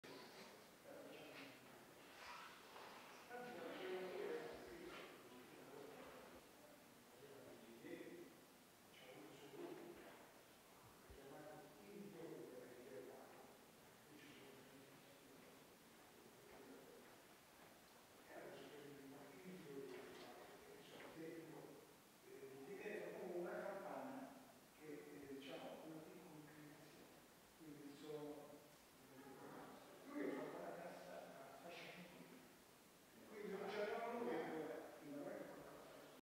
free stock footage / repertorio video immagini generiche / museo fisarmonica 2
immagini del museo della fisarmonica a castelfidardo nelle marche